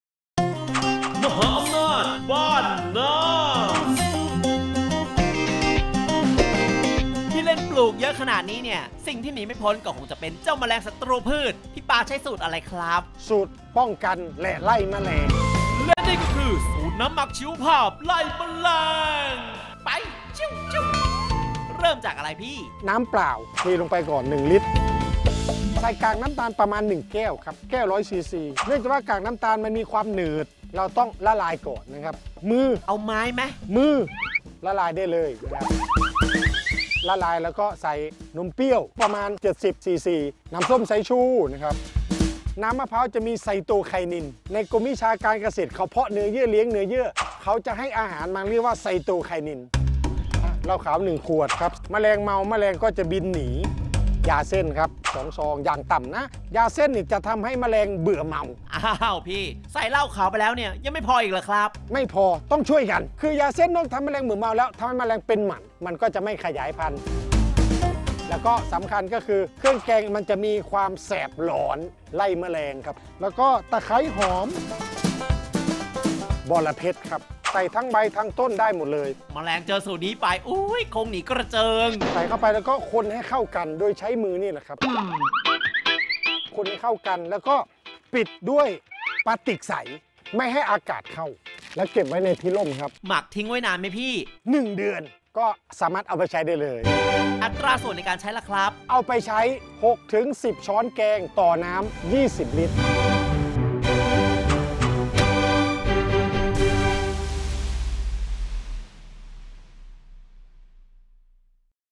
น้ำหมักชีวภาพไล่แมลง | สูตรลับฉบับบ้านนา
สูตรป้องกันและไล่แมลงของพี่ปา สามารถหาของที่มาทำน้ำหนักได้ง่ายมาก ๆ สูตรไม่ซับซ้อนไม่ว่าจะปลูกมากมายหลากหลายชนิดก็ตาม หากทำตามสูตรนี้แมลงไม่รอดอย่างแน่นอน
ติดตามสาระทางการเกษตรกับเกษตรกรตัวจริงได้ที่